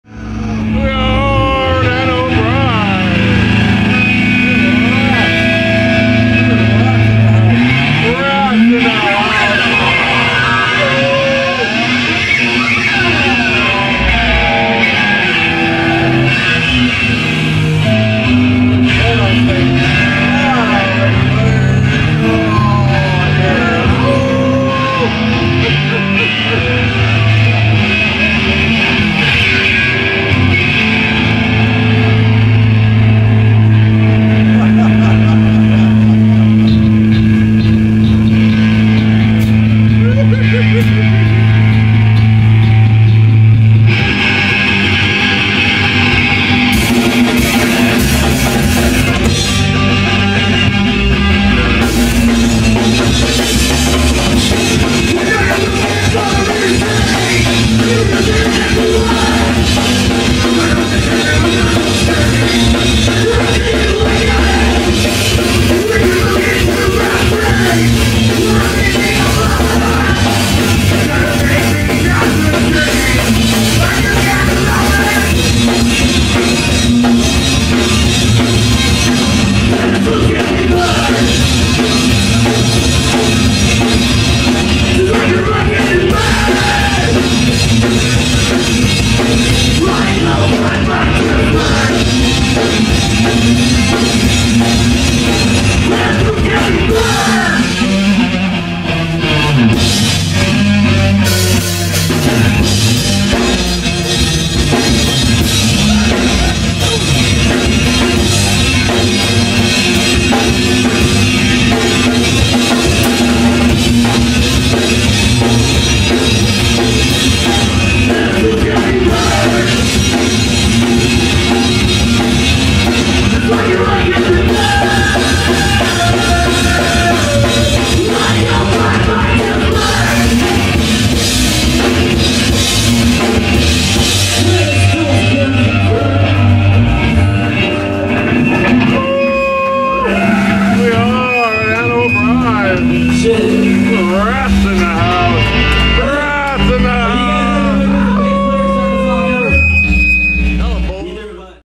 BIGDOGBRODAVE Presents RATS LIVE!!! 2024 Anna O'Briens Honolulu, Hawaii.
The angriest band in the islands doing their thing at Anna's.